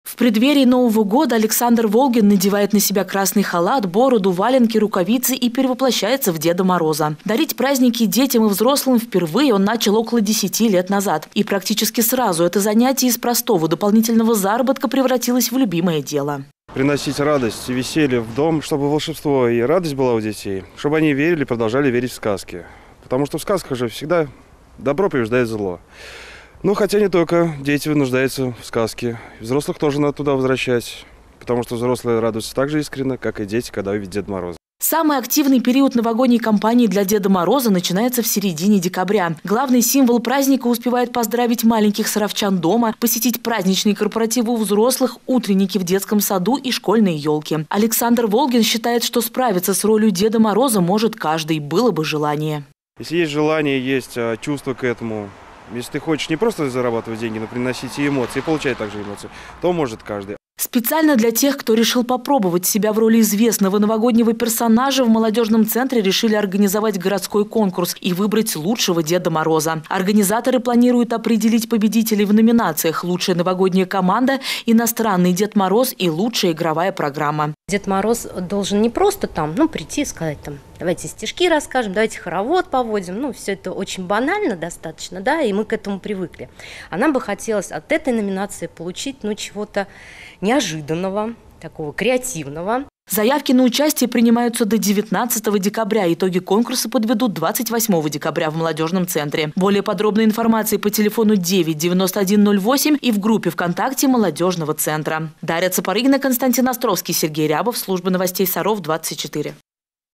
Конкурс "Лучший Дед Мороз"
В преддверии Нового года в Молодёжном центре решили провести городской конкурс и выбрать лучшего Деда Мороза. Победителей будут определять сразу в трёх номинациях.